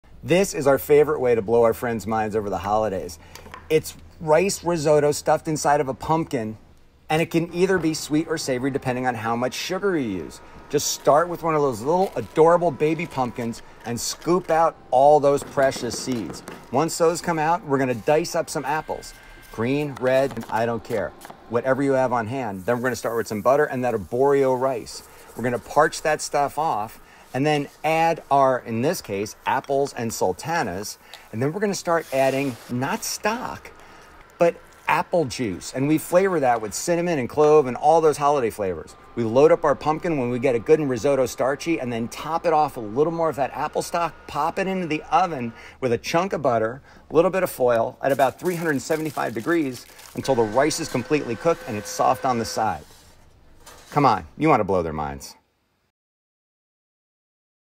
This is the EASIEST stuffed pumpkin recipe
This is the easiest stuffed pumpkin recipe! Follow along with Chef JP as he makes a fall classic. This pumpkin risotto can be made sweet or savory depending on how much sugar you add. Measure your seasonings with your heart!